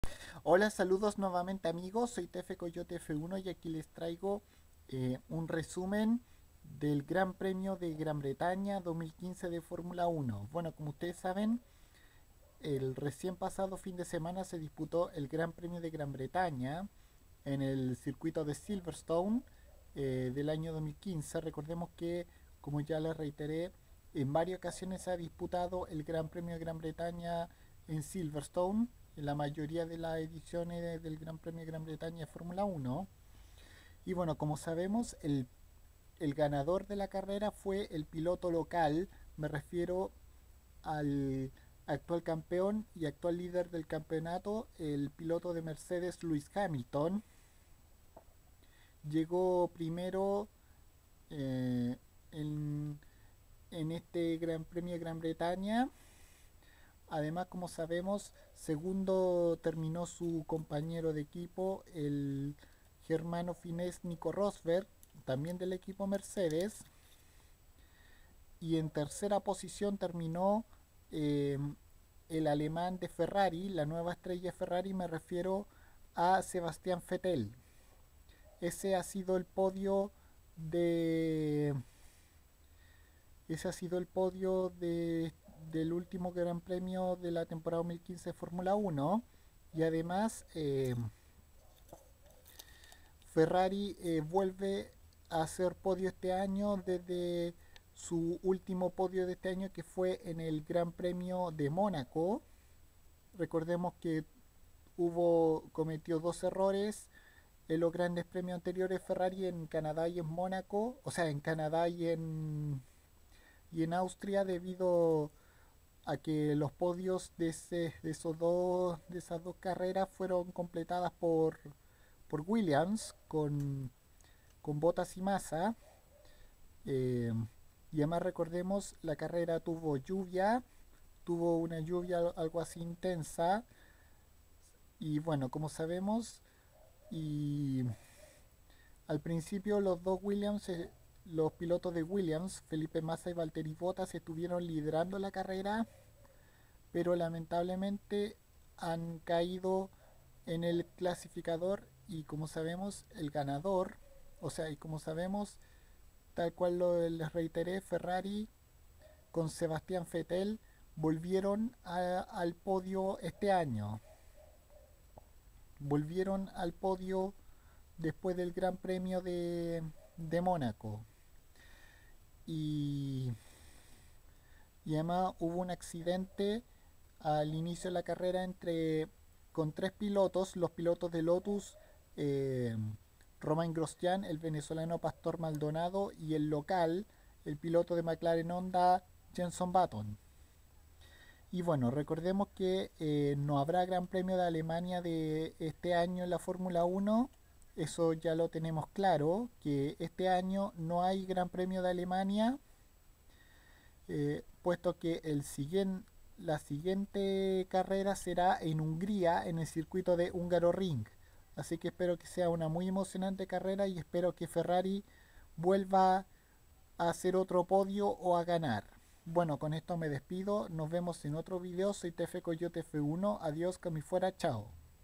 Resumen GP de Gran Bretaña 2015 de F1
Aquí un breve resumen de lo que fue el GP de Gran Bretaña 2015 de F1 el cual fue ganado por Lewis Hamilton,seguido de Nico Rosberg y Sebastian Vettel tercero

La próxima carrera será en Hungría el 26 de Julio en el Hungaroring